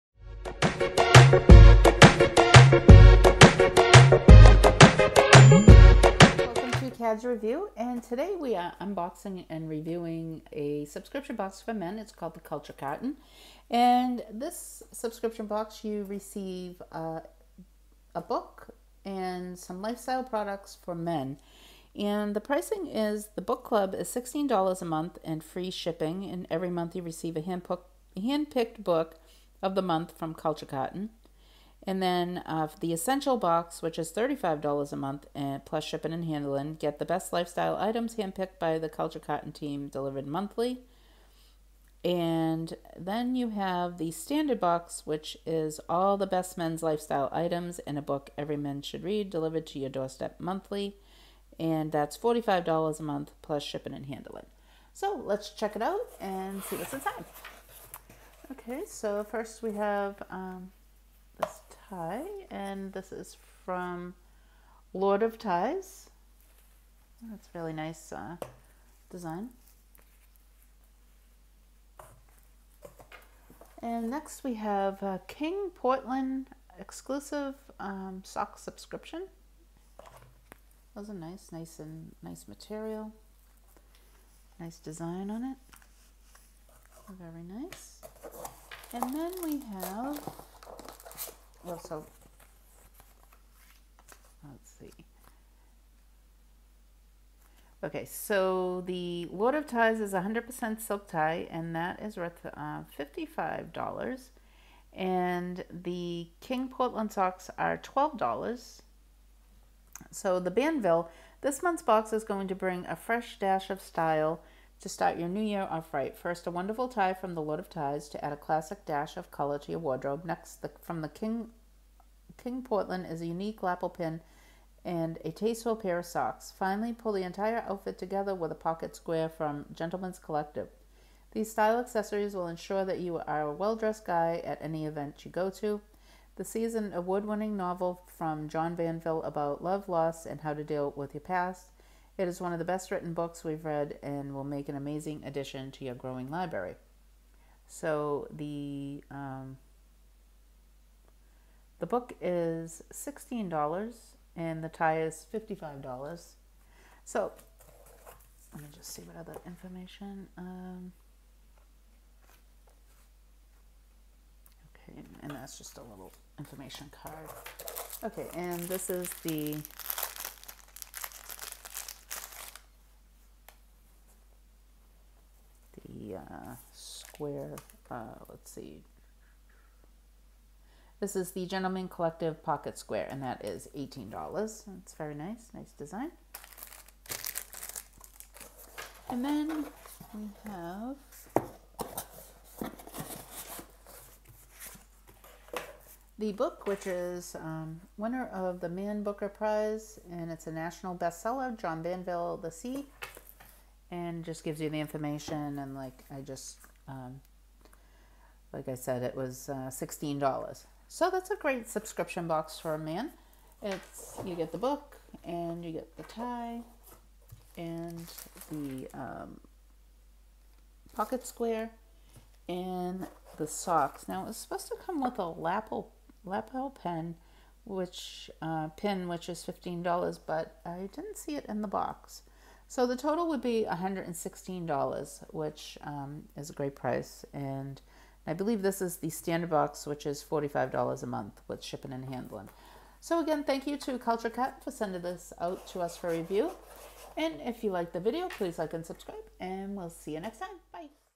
Culture Carton January (Unboxing)
Today we Unbox the Culture Carton Box. Thanks again for sending this out!